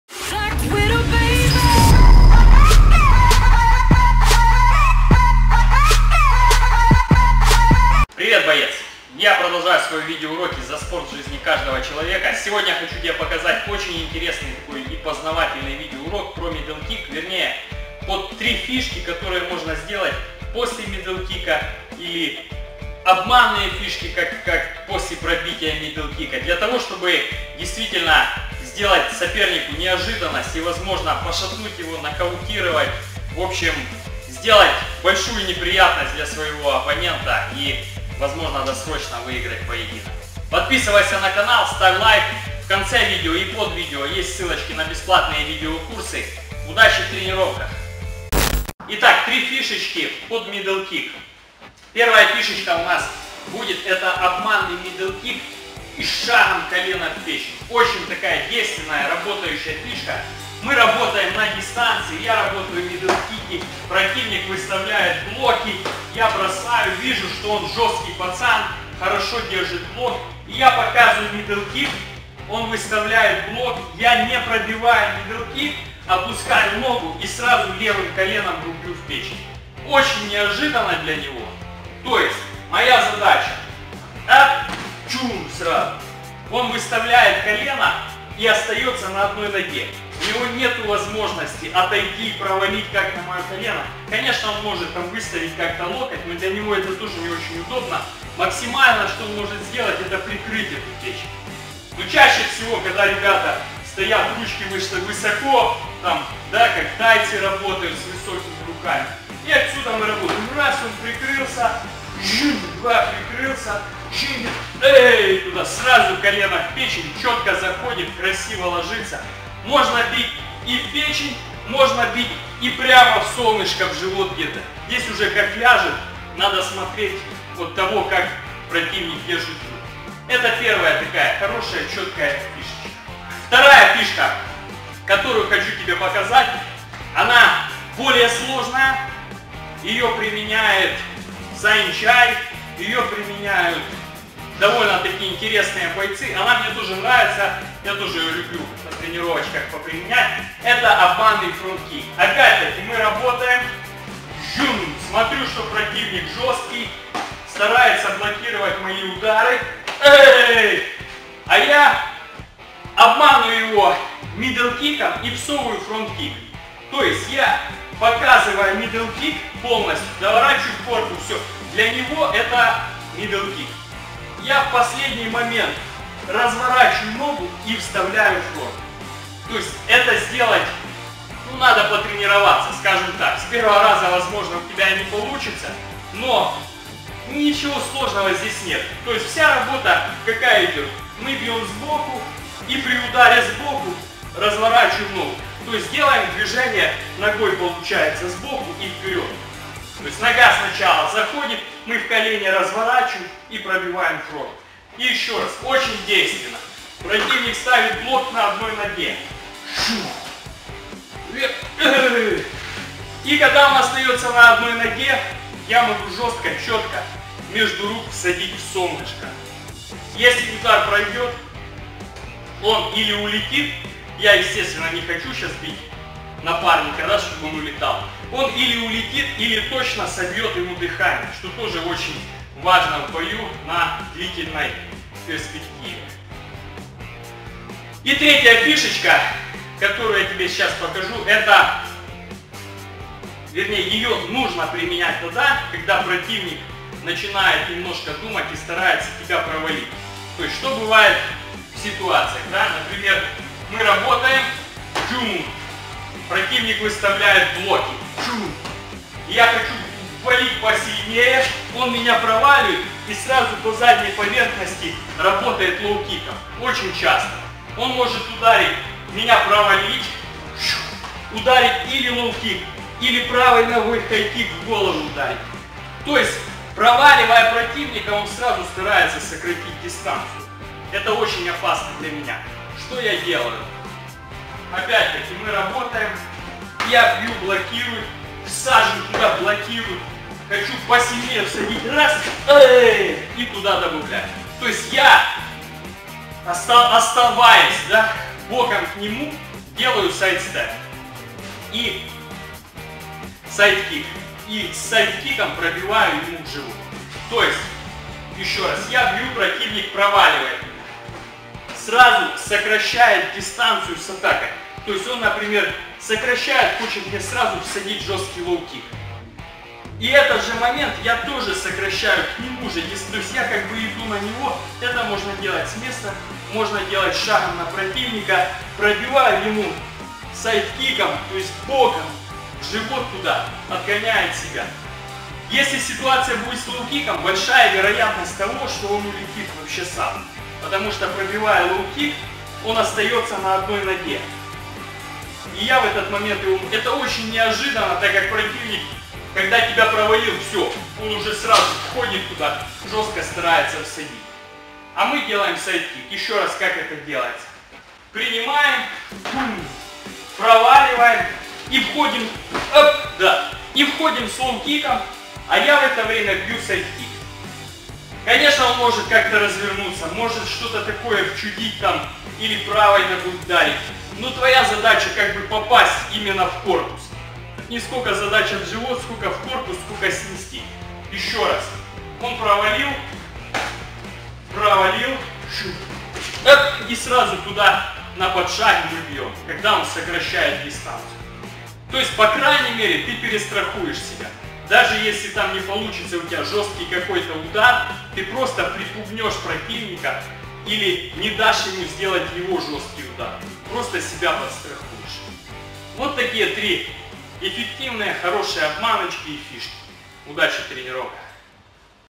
3 убойные фишки под мидл кик - уроки Муай Тай
В этом видео уроке по Муай Тай, я тебе покажу 3 убойных фишки под мидл кик, которые всегда проходят в боях!
50 пошагово расписанных тренировок, онлайн встречи и специальные бонусы в "Закрытом Онлайн клубе MUAYTHAICK"